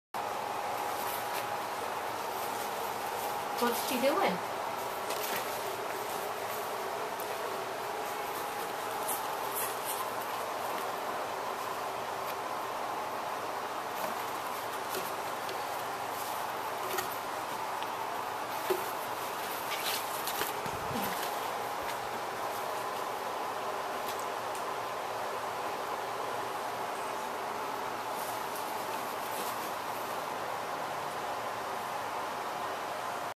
Trina Playing With Her Ribbon
Trina just playing with her ribbon and having a ball with it. No rude and mean comments allowed!! More vids coming soon.